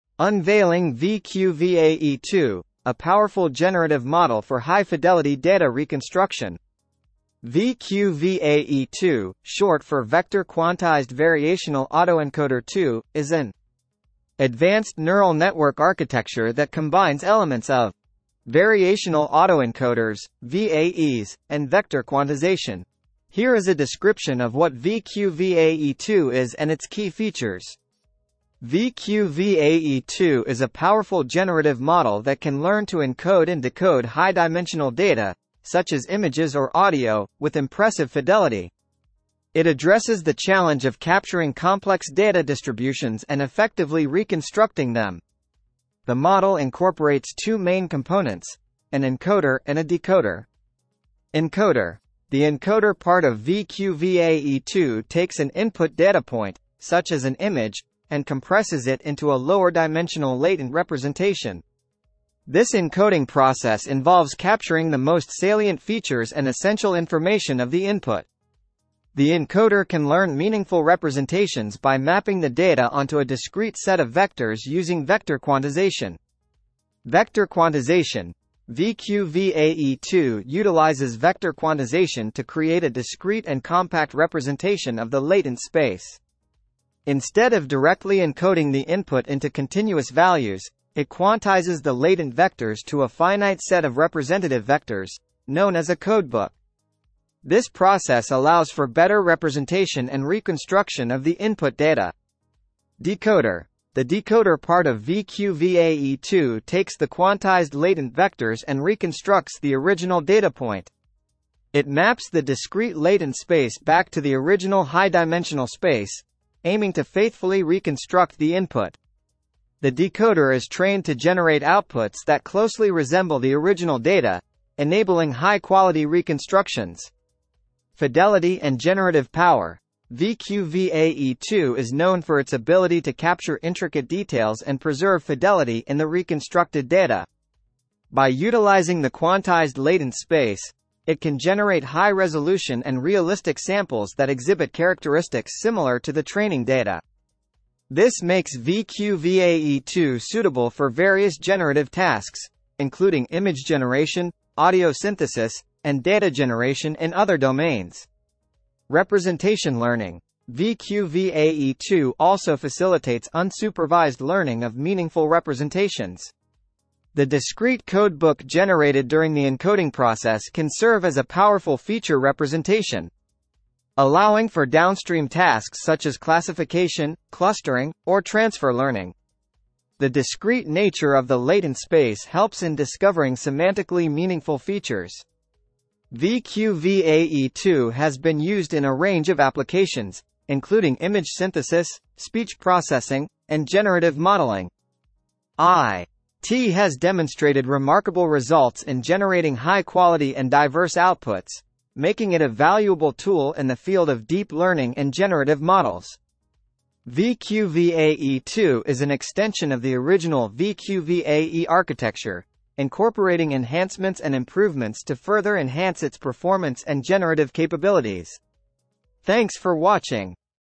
Unveiling VQ-VAE-2: A Powerful Generative Model for High-Fidelity Data Reconstruction
VQ-VAE-2, short for Vector Quantized Variational Autoencoder 2, is an advanced neural network architecture that combines elements of variational autoencoders (VAEs) and vector quantization. Here is a description of what VQ-VAE-2 is and its key features:

VQ-VAE-2 is a powerful generative model that can learn to encode and decode high-dimensional data, such as images or audio, with impressive fidelity. It addresses the challenge of capturing complex data distributions and effectively reconstructing them. The model incorporates two main components: an encoder and a decoder.

Encoder: The encoder part of VQ-VAE-2 takes an input data point, such as an image, and compresses it into a lower-dimensional latent representation. This encoding process involves capturing the most salient features and essential information of the input. The encoder can learn meaningful representations by mapping the data onto a discrete set of vectors using vector quantization.

Vector Quantization: VQ-VAE-2 utilizes vector quantization to create a discrete and compact representation of the latent space. Instead of directly encoding the input into continuous values, it quantizes the latent vectors to a finite set of representative vectors, known as a codebook. This process allows for better representation and reconstruction of the input data.

Decoder: The decoder part of VQ-VAE-2 takes the quantized latent vectors and reconstructs the original data point. It maps the discrete latent space back to the original high-dimensional space, aiming to faithfully reconstruct the input. The decoder is trained to generate outputs that closely resemble the original data, enabling high-quality reconstructions.

Fidelity and Generative Power: VQ-VAE-2 is known for its ability to capture intricate details and preserve fidelity in the reconstructed data. By utilizing the quantized latent space, it can generate high-resolution and realistic samples that exhibit characteristics similar to the training data. This makes VQ-VAE-2 suitable for various generative tasks, including image generation, audio synthesis, and data generation in other domains.

Representation Learning: VQ-VAE-2 also facilitates unsupervised learning of meaningful representations. The discrete codebook generated during the encoding process can serve as a powerful feature representation, allowing for downstream tasks such as classification, clustering, or transfer learning. The discrete nature of the latent space helps in discovering semantically meaningful features.

VQ-VAE-2 has been used in a range of applications, including image synthesis, speech processing, and generative modeling. It has demonstrated remarkable results in generating high-quality and diverse outputs, making it a valuable tool in the field of deep learning and generative models.

Note: VQ-VAE-2 is an extension of the original VQ-VAE architecture, incorporating enhancements and improvements to further enhance its performance and generative capabilities.